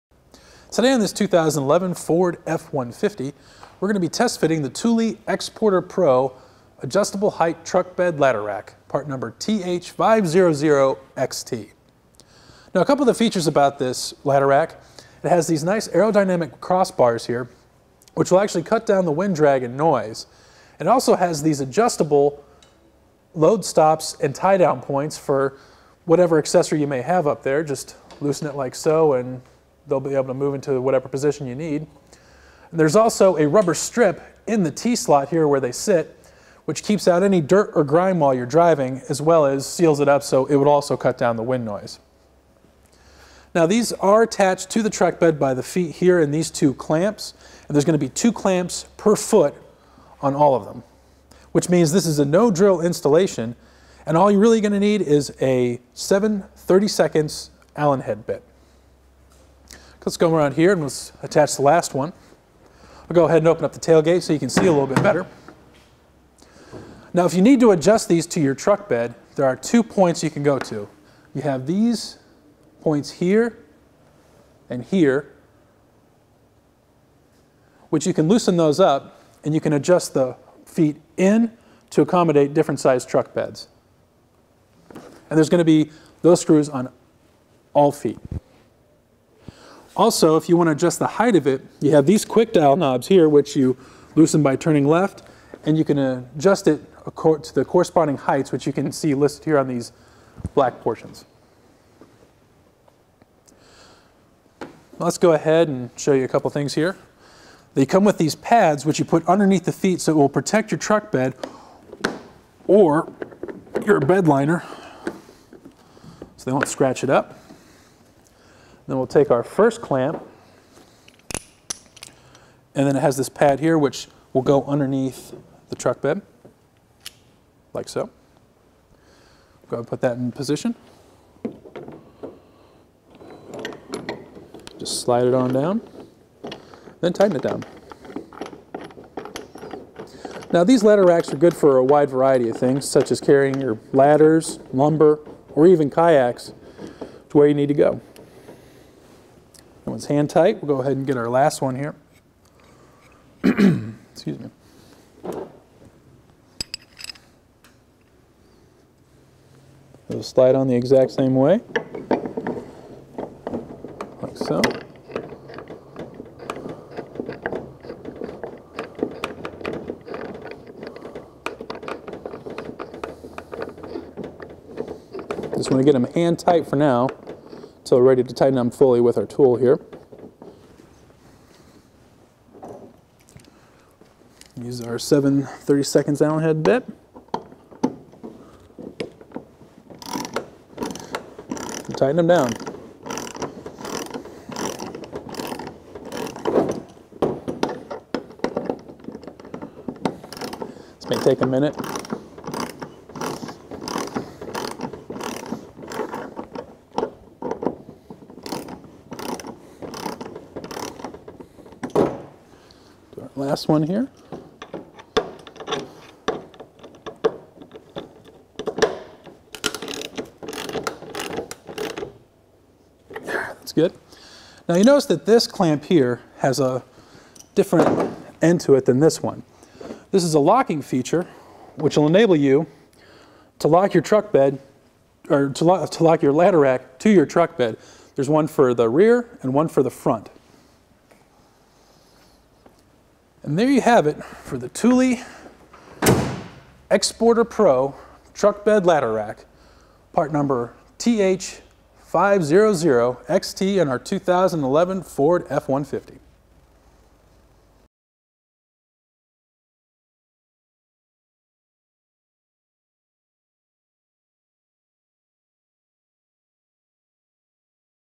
Fit Check: Testing Thule Xsporter Pro Adjustable Height Truck Bed Ladder Rack on a 2011 Ford F-150
Today on this 2011 Ford F150 we're going to be test fitting the Thule Exporter Pro adjustable height truck bed ladder rack, part number TH500XT. a couple of the features about this ladder rack. It has these nice aerodynamic crossbars here which will actually cut down the wind drag and noise. It also has these adjustable load stops and tie down points for whatever accessories you may have up there. Just loosen it like so and they'll be able to move into whatever position you need. There's also a rubber strip in the T slot here where they sit which keeps out any dirt or grime while you're driving, as well as seals it up so it would also cut down the wind noise. These are attached to the truck bed by the feet here, and these 2 clamps. There's going to be 2 clamps per foot on all of them, which means this is a no drill installation, and all you're really going to need is a 7/32 Allen head bit.

Let's go around here and let's attach the last one. I'll go ahead and open up the tailgate so you can see a little bit better. If you need to adjust these to your truck bed there are 2 points you can go to. There are these points here and here, which you can loosen those up and you can adjust the feet in to accommodate different size truck beds. There's going to be those screws on all feet.

Also, if you want to adjust the height of it, you have these quick dial knobs here, which you loosen by turning left, and you can adjust it to the corresponding heights which you can see listed here on these black portions. Let's go ahead and show you a couple things here. They come with these pads which you put underneath the feet so it will protect your truck bed or your bed liner so they won't scratch it up. Then we'll take our first clamp, and it has this pad here which will go underneath the truck bed like so. We'll go ahead and put that in position. Just slide it on down, then tighten it down.

These ladder racks are good for a wide variety of things such as carrying your ladders, lumber, or even kayaks to where you need to go. That one's hand tight. We'll go ahead and get our last one here. Excuse me. It'll slide on the exact same way, like so.

Just want to get them hand tight for now so we're ready to tighten them fully with our tool here. Use our 7/32 Allen head bit to tighten them down. This may take a minute. Do our last one here. It's good. Now you notice that this clamp here has a different end to it than this one. This is a locking feature which will enable you to lock your ladder rack to your truck bed. There's one for the rear and one for the front. There you have it for the Thule Exporter Pro truck bed ladder rack, part number TH500XT on our 2011 Ford F150.